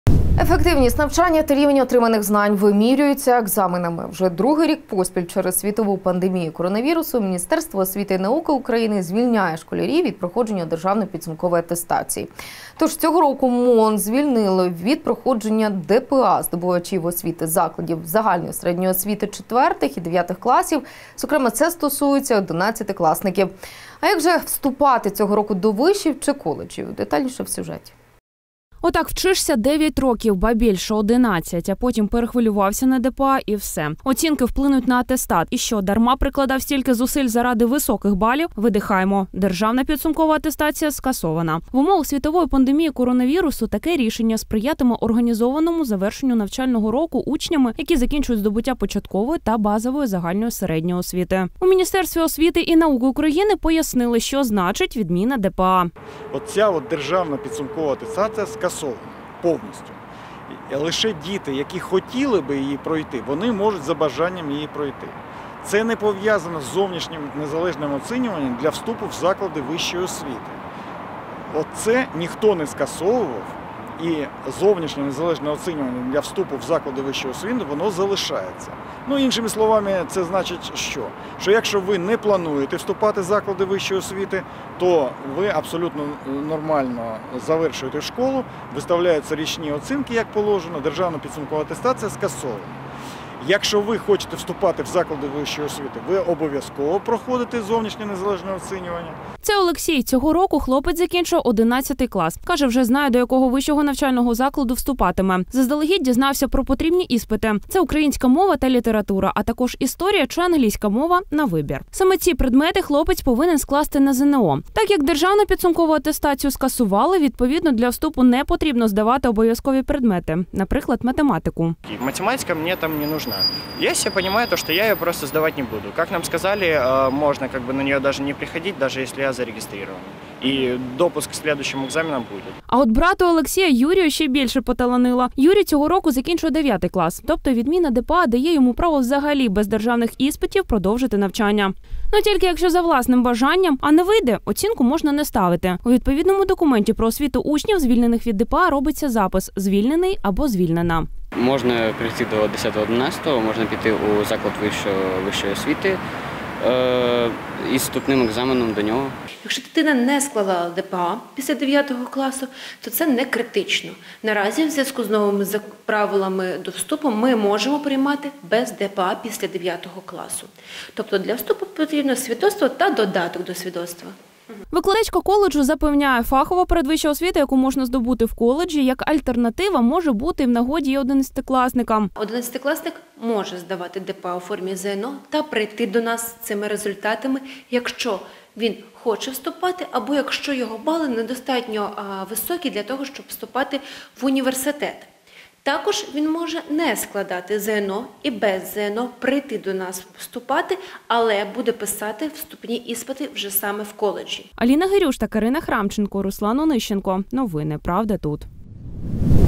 МОН СКАСУВАЛО ДЕРЖАВНУ ПІДСУМКОВУ АТЕСТАЦІЮ
Верховна Рада 13 квітня підтримала законопроєкт, який скасовує проходження обов'язкової державної підсумкової атестації для учнів у 2021 році. Таке рішення МОН України дозволяє випускникові школи, студентові коледжу чи ПТУ самостійно вирішувати, чи проходити ДПА у формі ЗНО цього року. Відміна ДПА для 4 класу чи 9 класу дає право школярам вільно переходити до наступного рівня середньої-загальної освіти.